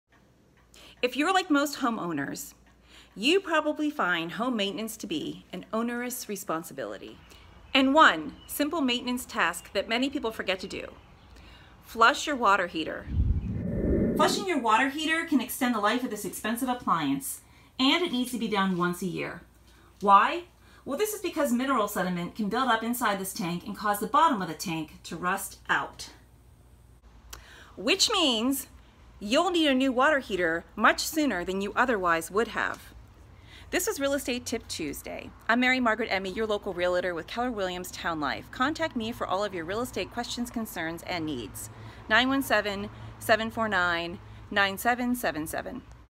Real Estate Tip Tuesday! how are YOU with home maintenance?
In this Real Estate Tip Tuesday, I explain the importance of draining your hot water heater tank once a year. Now that we are all in quarantine, what better time to do this important home maintenance!